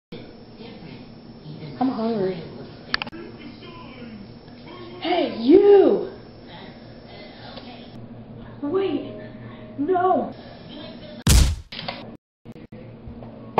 The Sandwich - A Short Film
A Short Test Film For Muzzle Flashes And Blood Splatter FX. Tutorials Coming Soon!